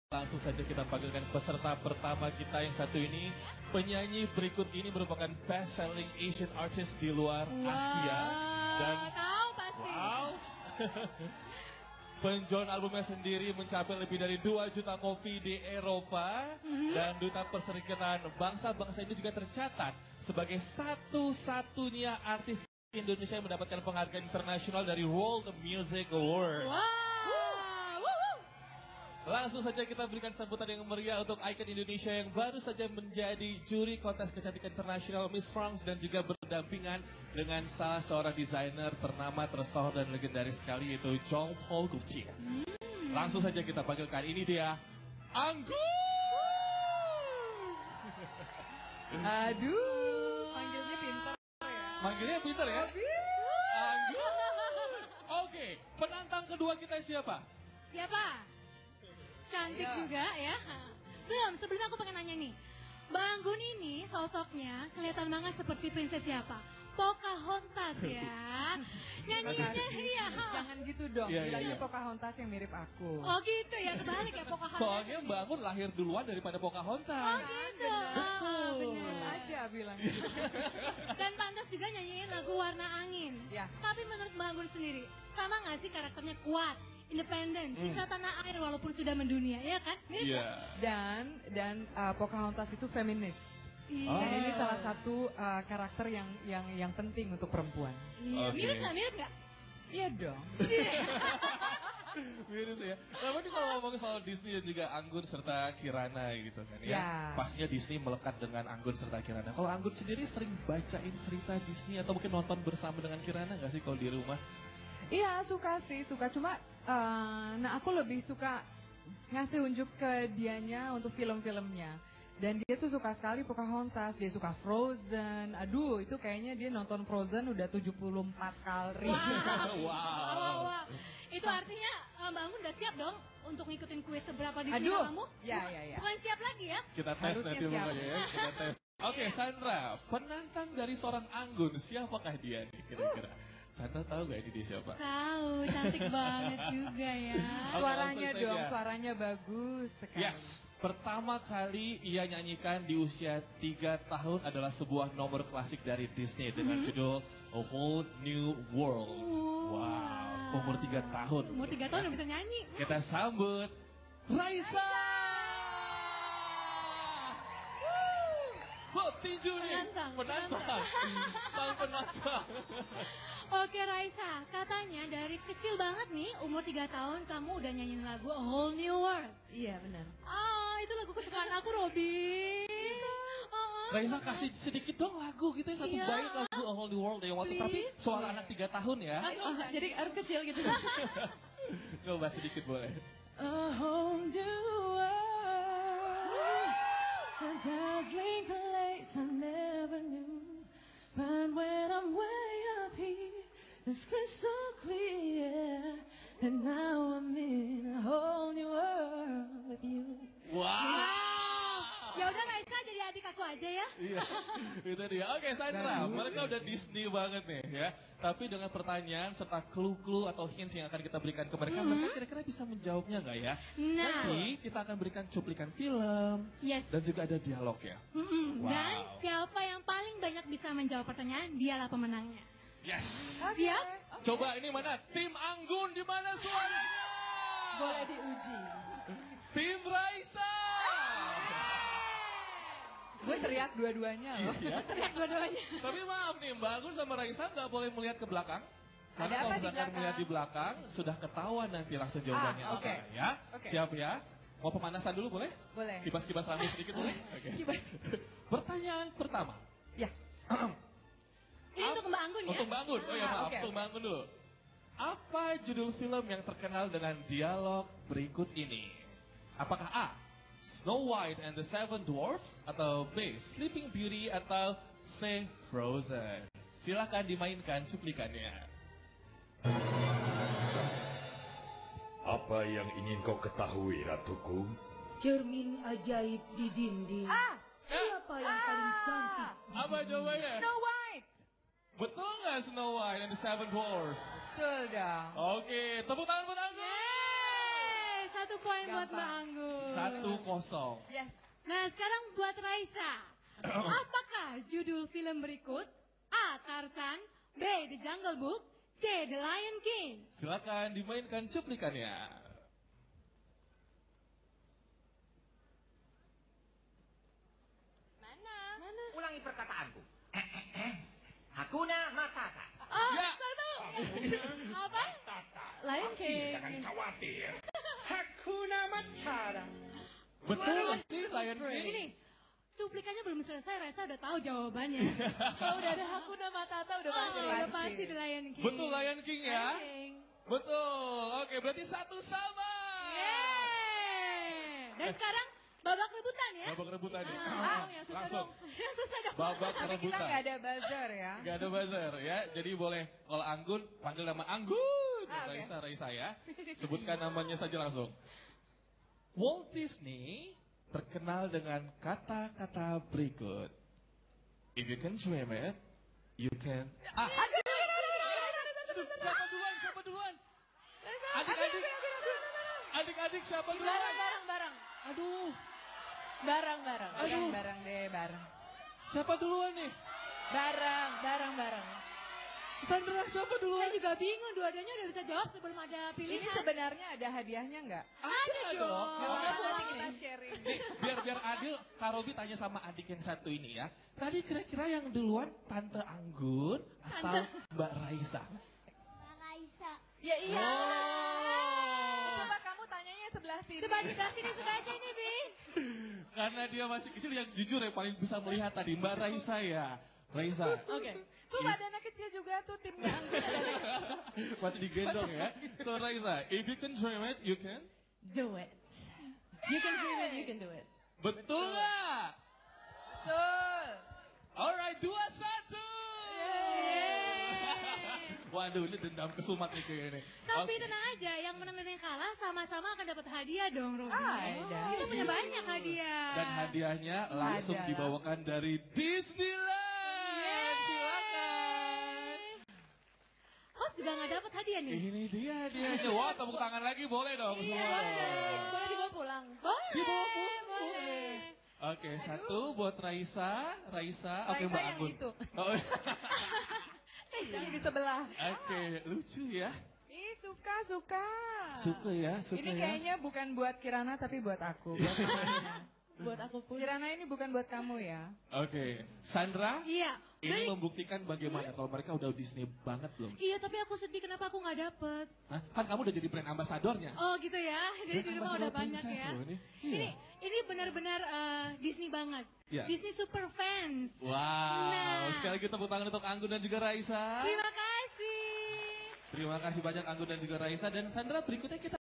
Q&A Anggun and Raisa at We Love Disney Concert
Anggun and Raisa were asked about their knowledge about Disney by Robby Purba and Sandra Dewi at We Love Disney Concert, on December 23rd, 2015. They both got a souvenir from Disneyland.

Wanna show your love to Anggun? Always buy her original CDs and download them legally!